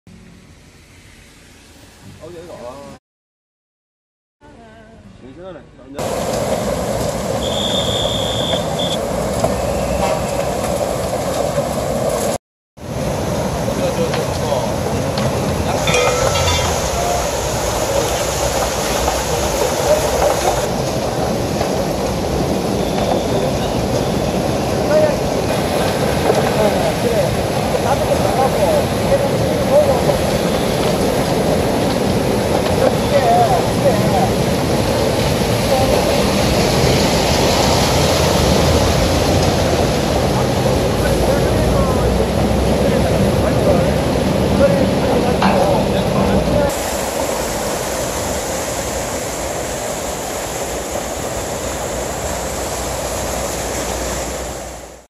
[경상일보TV]두왕사거리 관로 파열로 인해 물에 잠기는 사고 발생
15일 울산시 남구 두왕사거리에서 상수도관이 파열되는 사고가 발생해 일대 도로가 물바다로 변해있다.